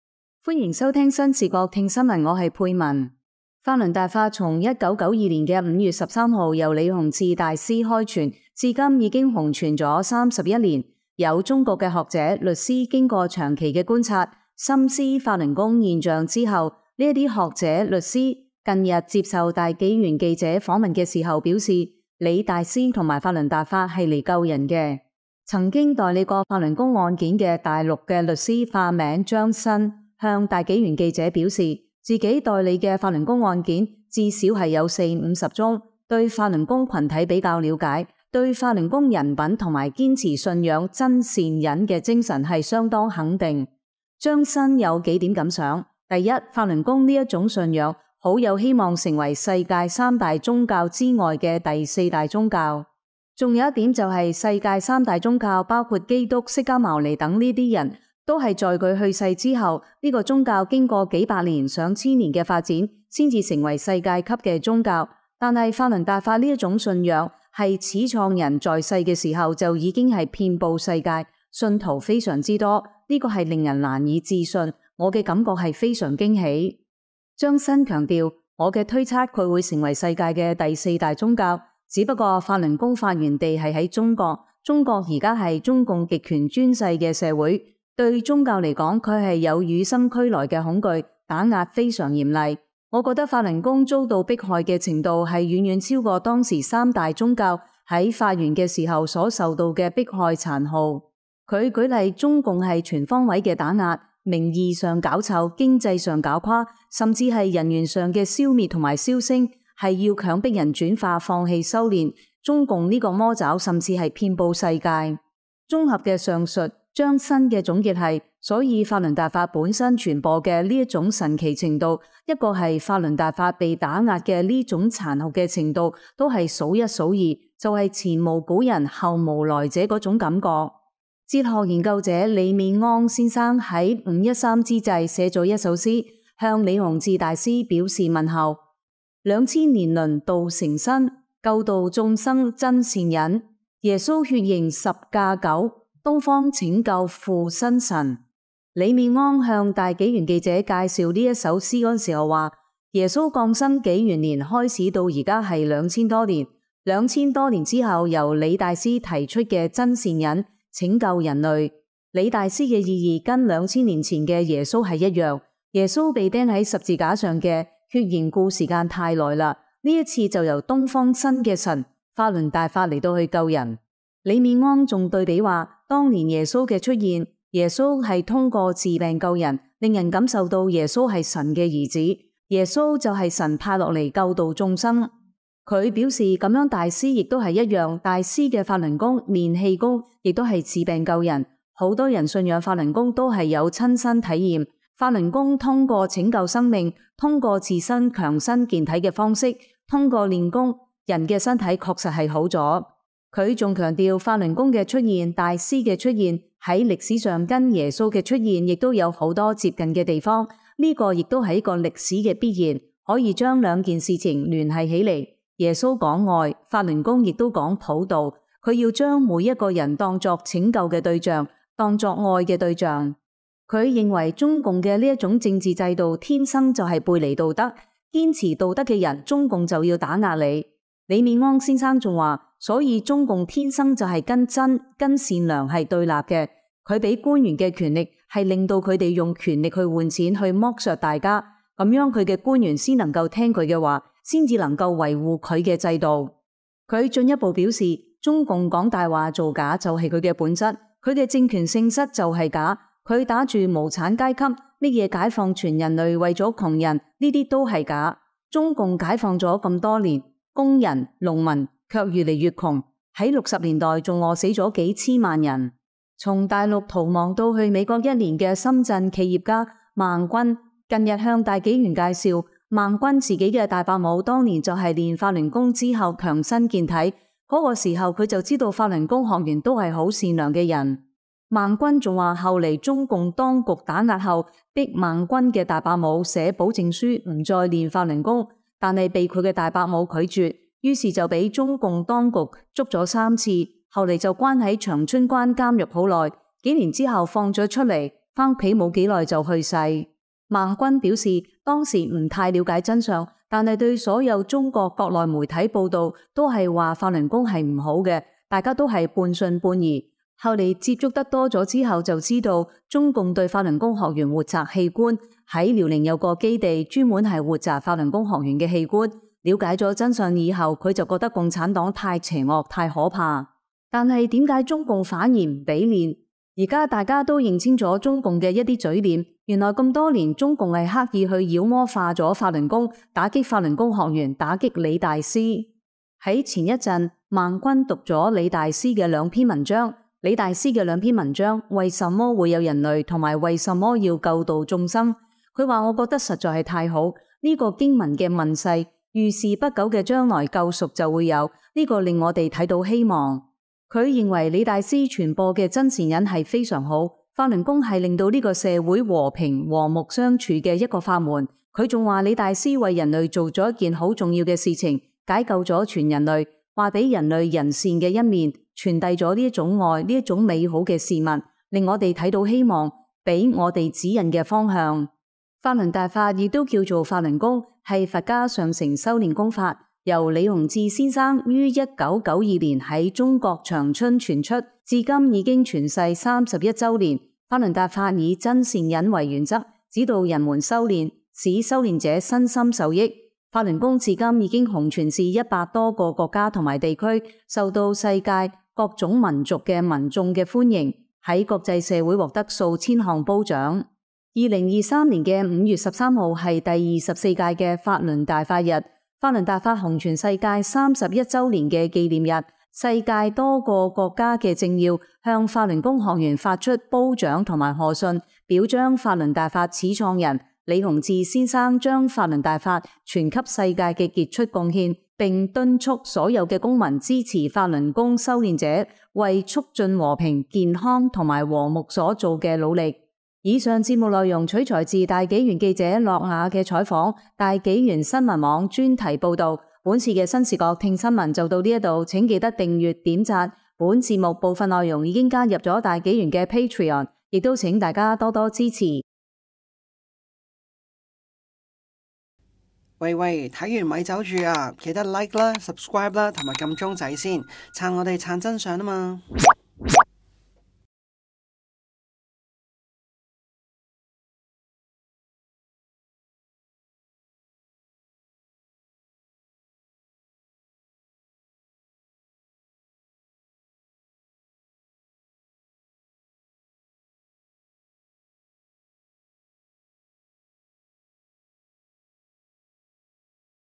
法輪大法自1992年5月13日由李洪志大師開傳至今，已洪傳31年，已有多名中國學者、律師等深思法輪功現象，近日接受大紀元記者採訪表示，李大師和法輪大法是來救人的。
大法洪傳31周年  中國學者律師深思法輪功現象

新紀元5月刊【ChatGPT「天性」會撒謊】：📖現已上市🎉